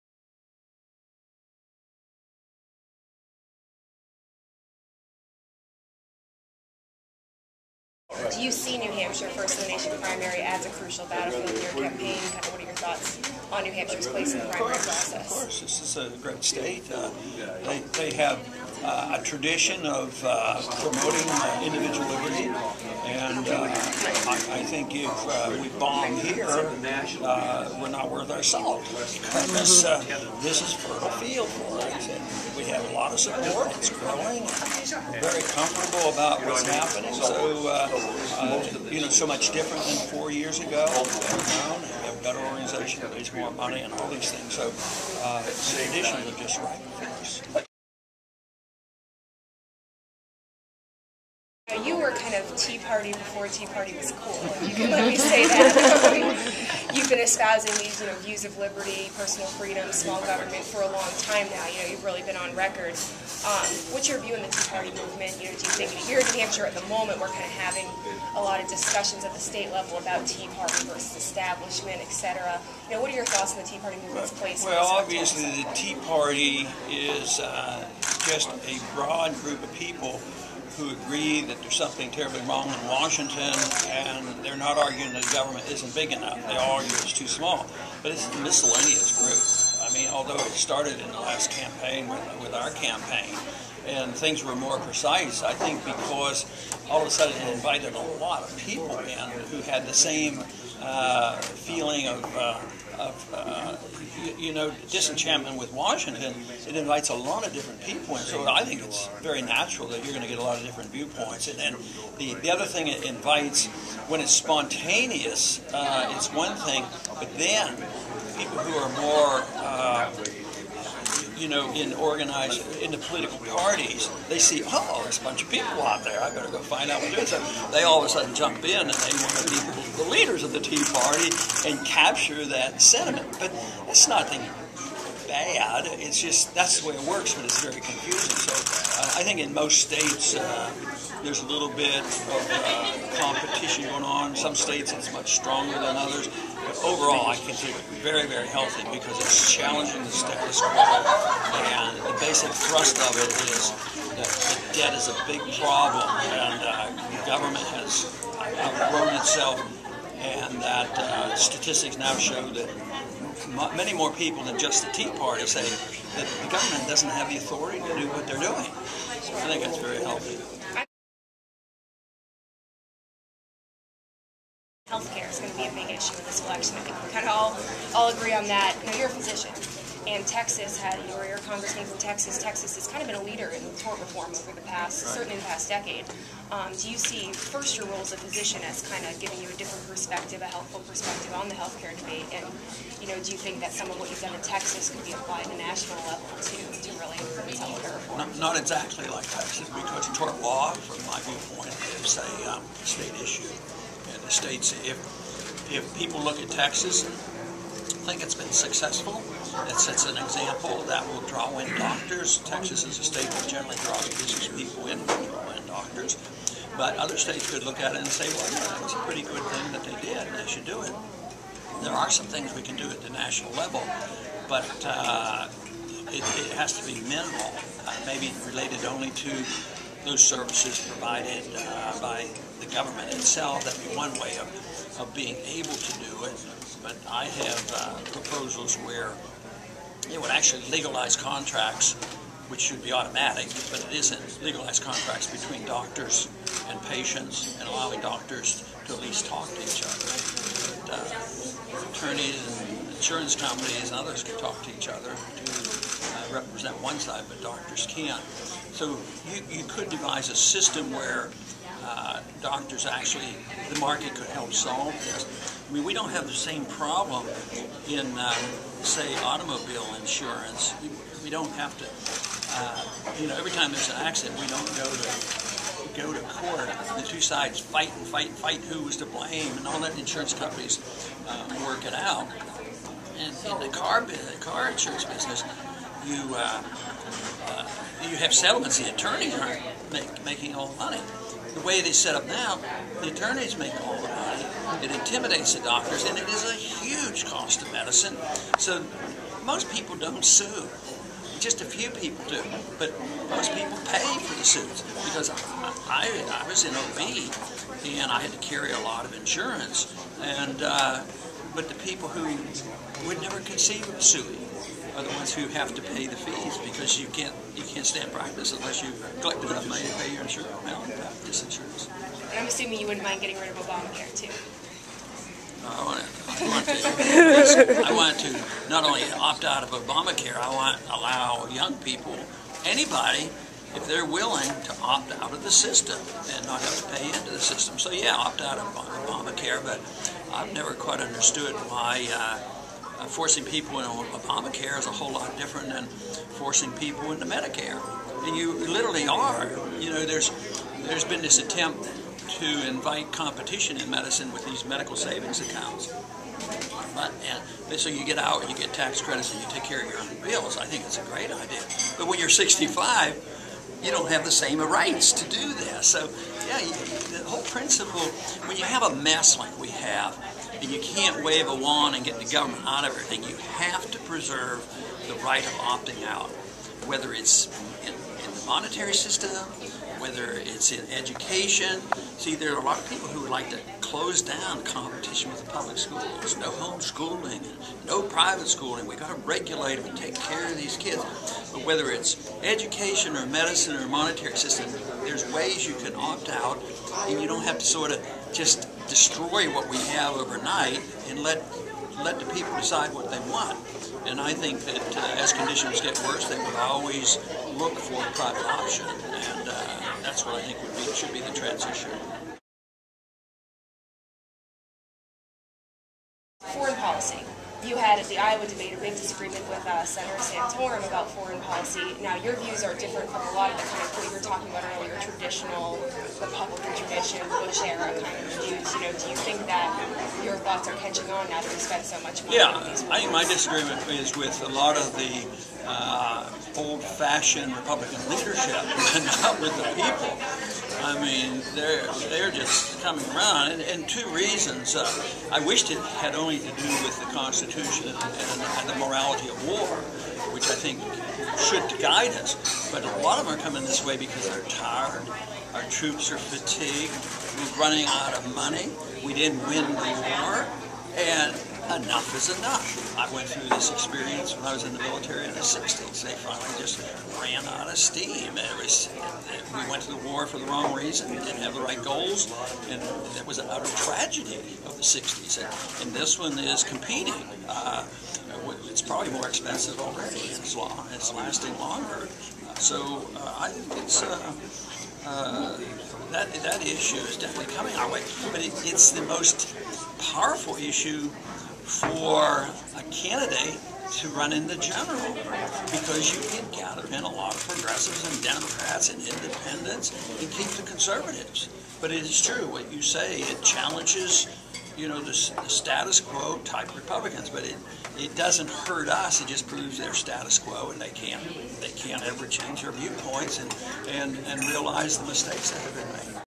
NH Journal Interviews Ron Paul
Texas Congressman and Presidential candidate Ron Paul talks with NH Journal's Amelia Chasse about health care, foreign policy, the Tea Party, and the First-in-the-Nation Primary